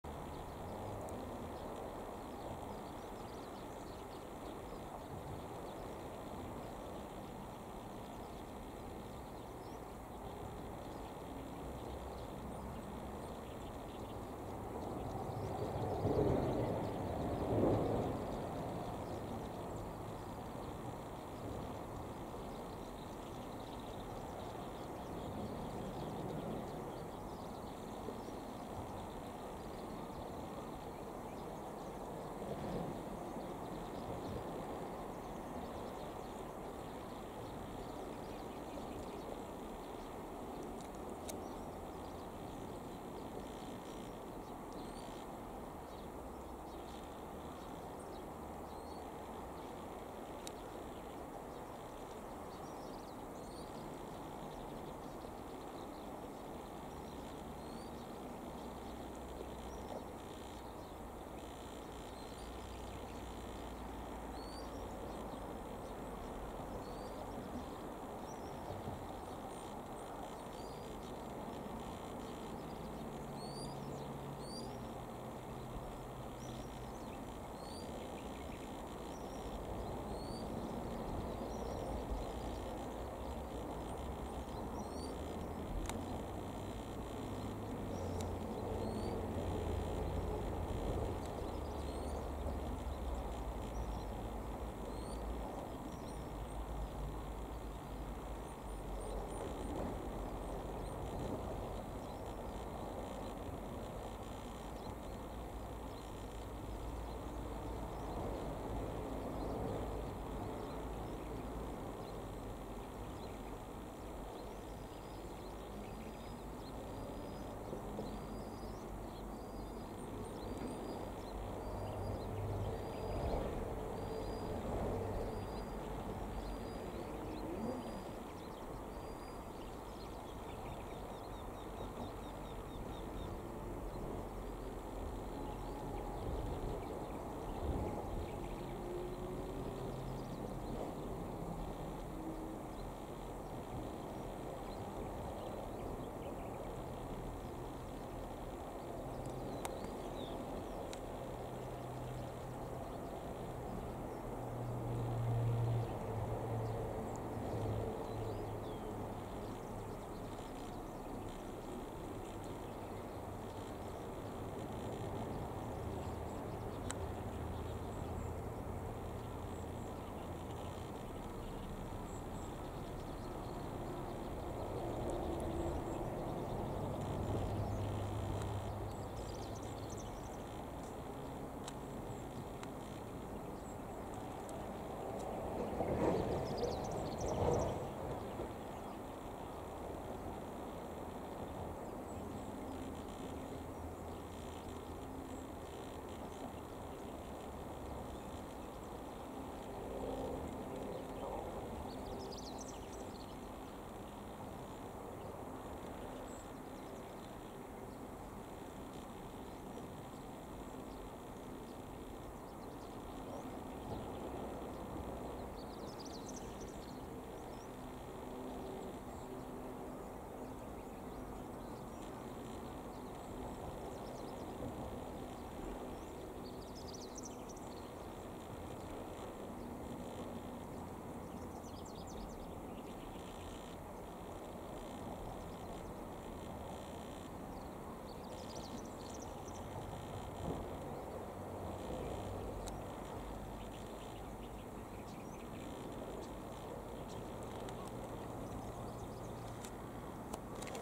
イソシギの親子
画質が悪くて大変申し訳ございません。体格の良いイソシギかと思いきや、４羽の雛を包囲してたんですね。ママの優しさが微笑ましいです。